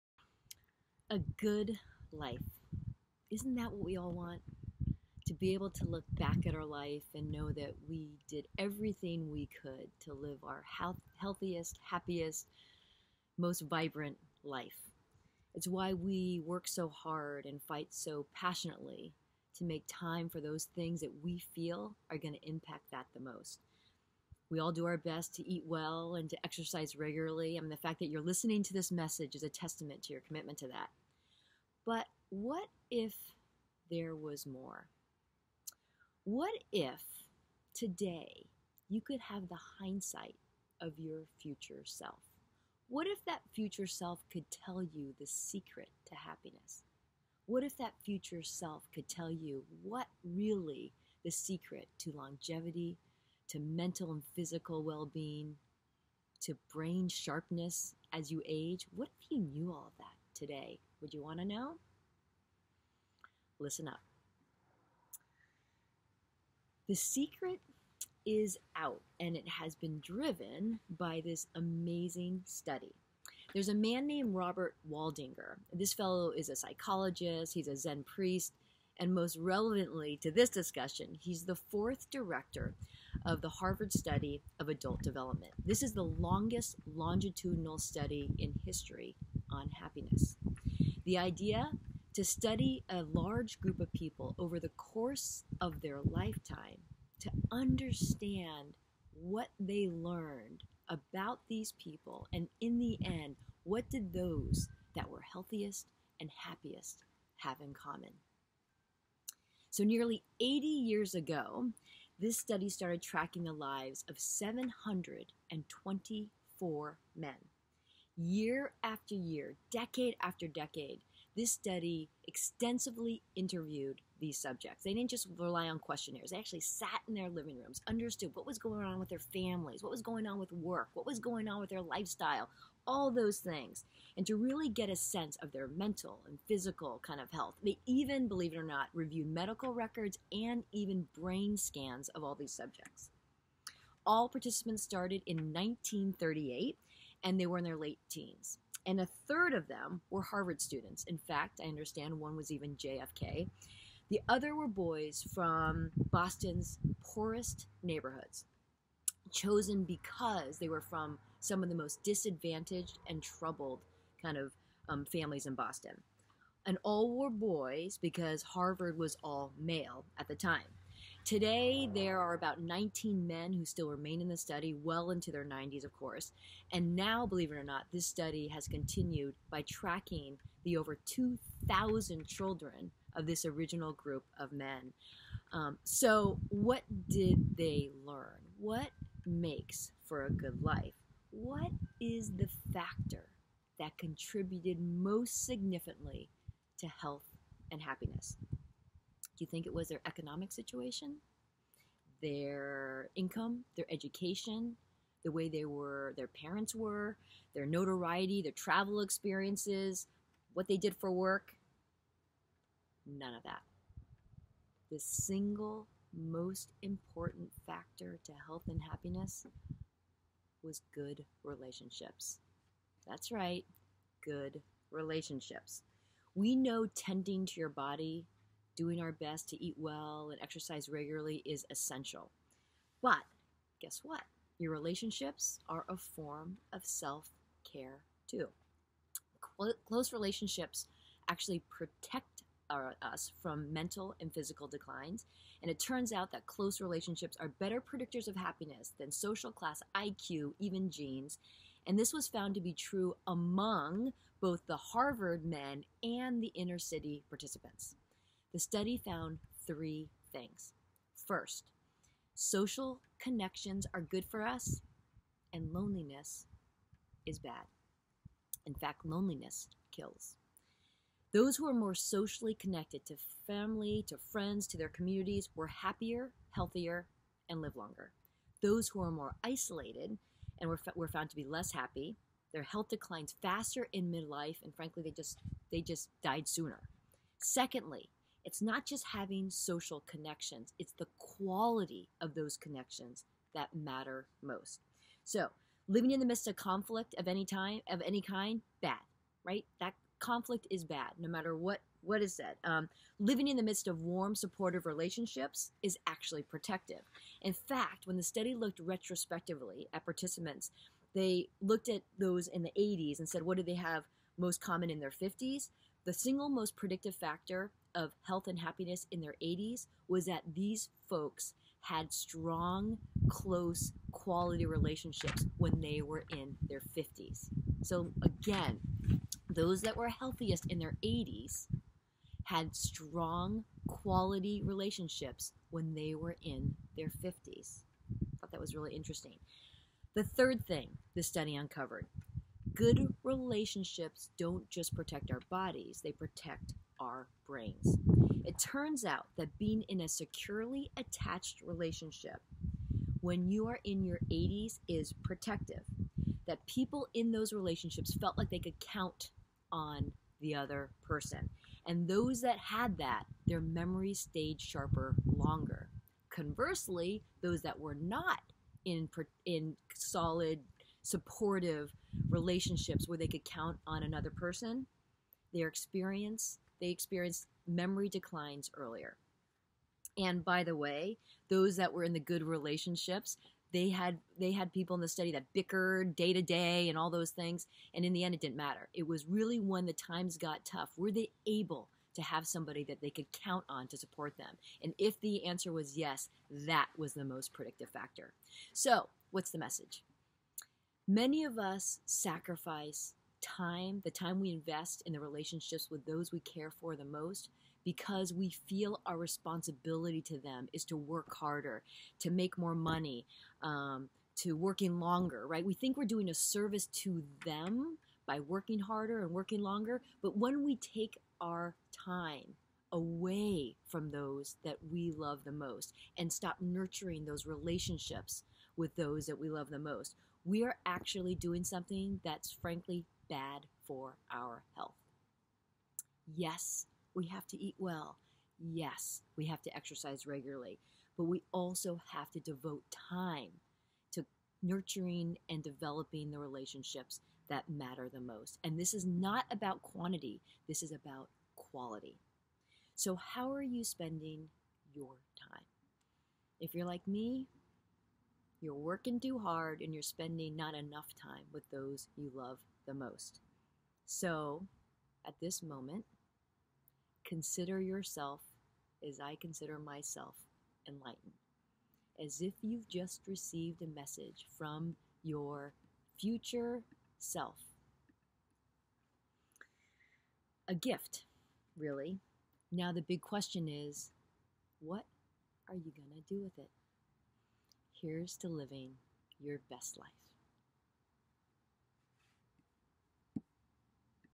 Mary Obana: The Secret to a Happy Life
An 80-year-long study on happiness finds that its more than money, genes or physical appearance that makes for a good life. It's connection.